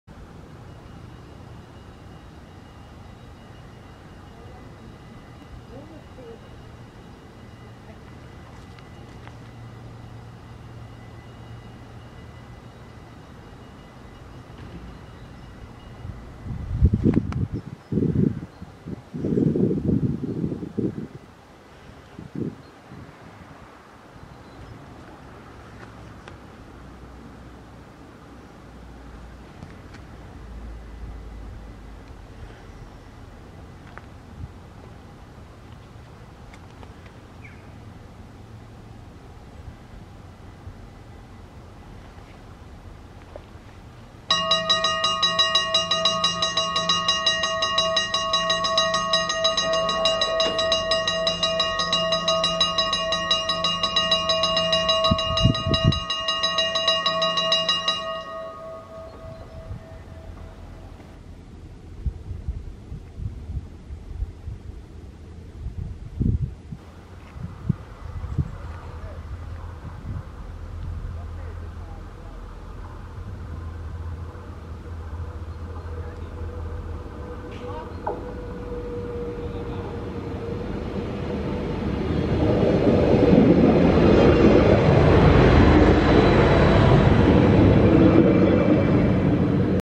Arriva GTW-D 279 arriveert te Cuijk!
Arriva GTW-D 279 in de Maaslijn kleuren komt aan op station Cuijk, als veel te korte stoptrein naar Roermond.